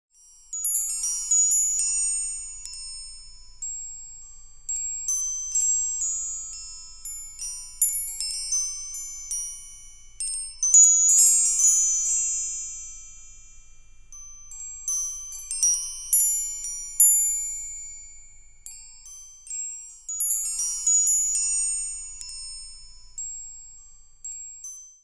Woodstock Crystal Meditation Chime - Black by Woodstock Chimes
This windchime's crystal accents and soothing sounds can be a focal point for meditation. Black rods give it a cool, contemporary look.

This chime can be used as an aid to meditation as a musical and visual mantra to help relax the mind and focus one's thoughts. Or enjoy it anytime for its soothing sounds and beautiful crystal accents. The practice of meditation has been an integral part of many religions for thousands of years. It has also been adopted, in modern times, as a way to improve physical and mental well-being. There are many different meditation techniques, but whether you are seeking spiritual growth or simply a little stress relief, it is important to create the right environment. Let our Crystal Meditation Chime help you create just the right ambience.